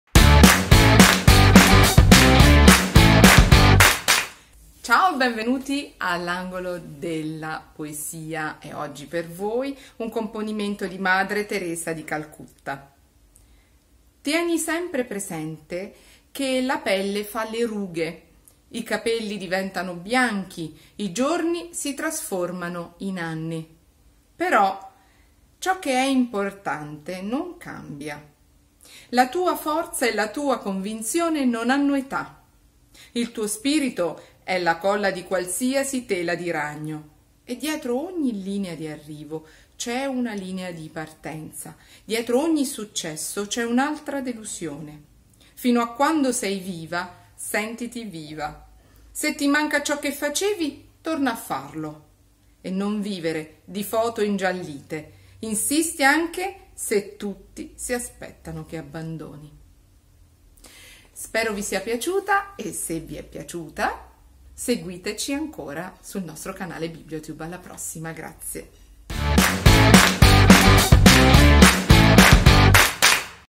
"Tieni sempre presente" di Madre Teresa di Calcutta
tema: trascorrere del tempo, elogio all'impegno delle donne
età: 14+
Blog -  BIBLIOG: A CACCIA DI LIBRI
Vi aspettiamo!

per info e prenotazioni: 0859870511